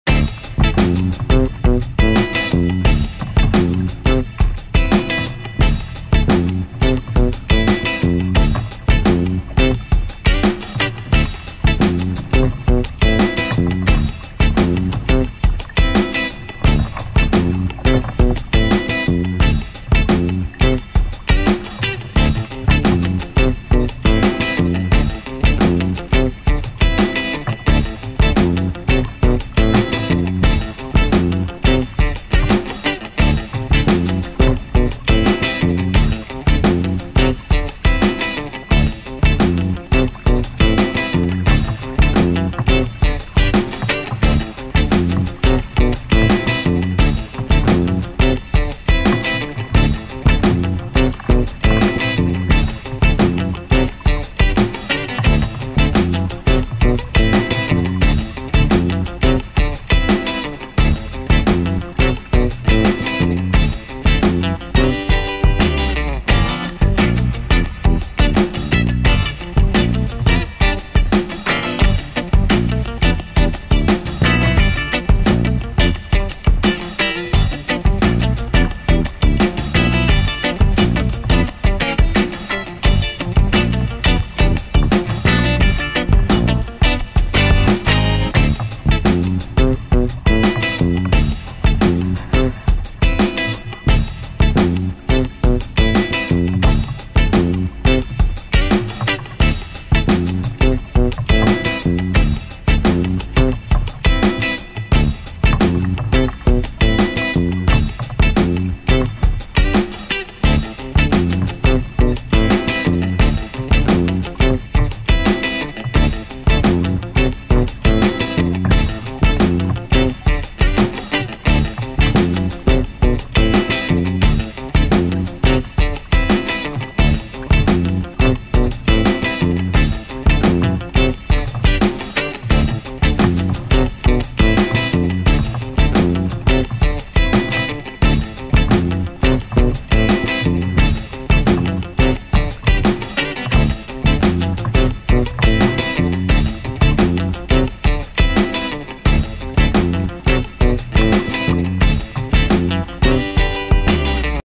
Moyo my Rhodesian Ridgeback puppy first time kayaking.
My Moyo Rhodesian Ridgeback first day kayaking.